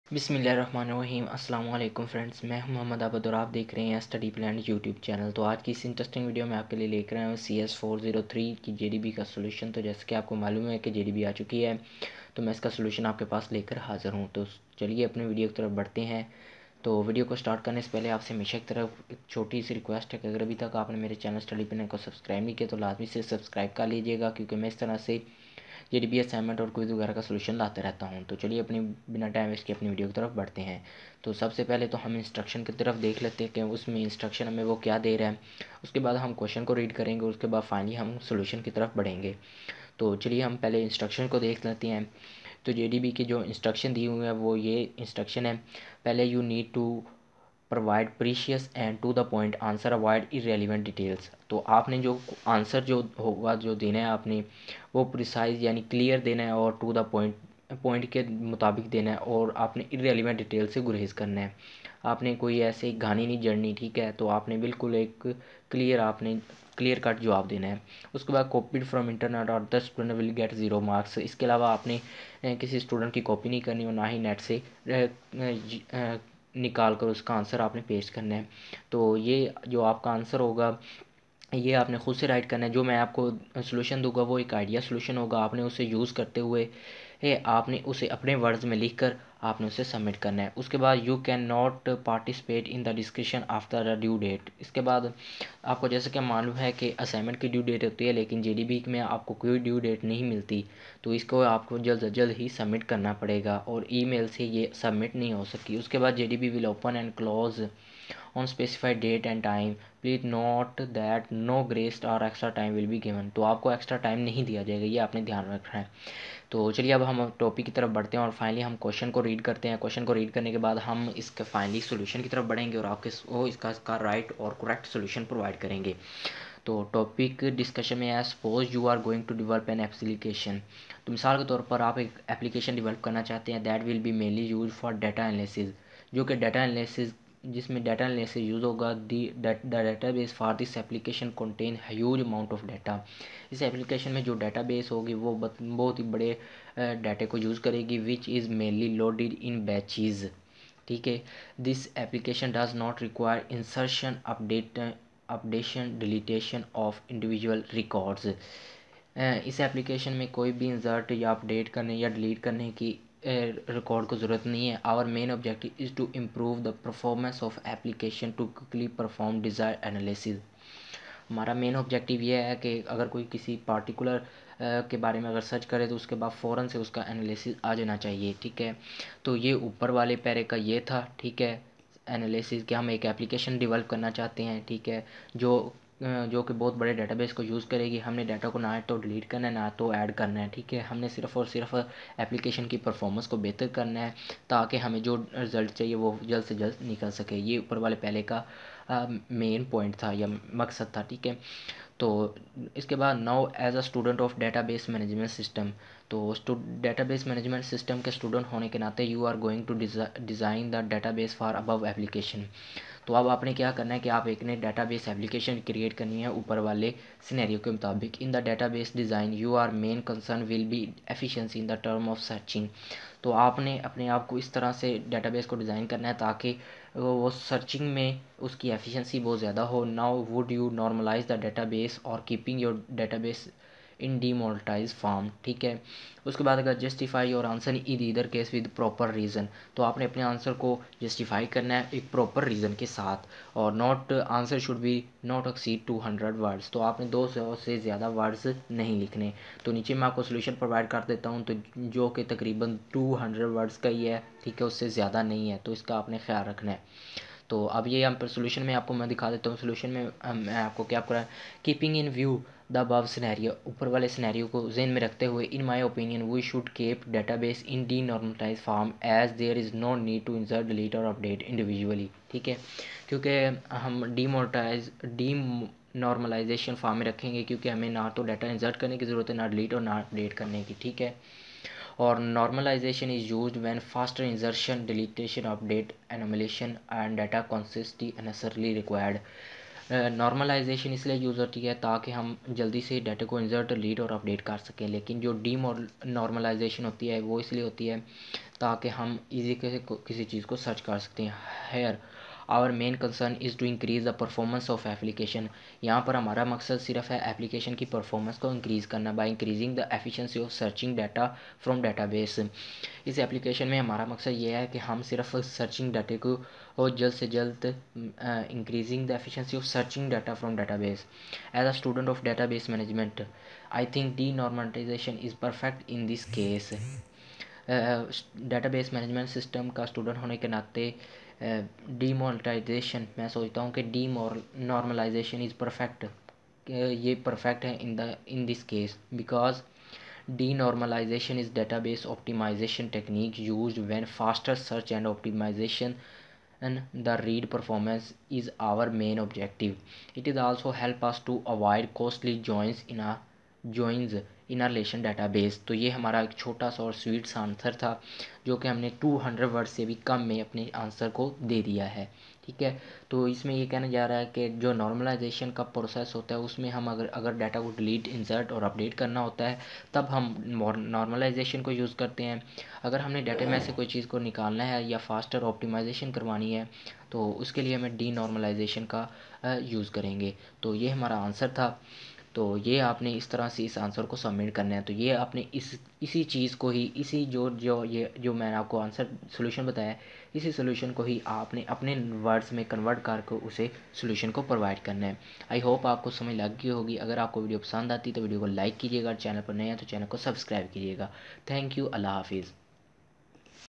CS403 GDB Solution Fall 2019 & 2020 | Study Planet
Hi! Guys
Study Planet

In this video i will tell u CS403 GDB Solution Fall 2019 & 2020  | Study Planet.

How To Copy VU Quiz And Search On Google | VU Quiz Allow Copy | Technical Urdu New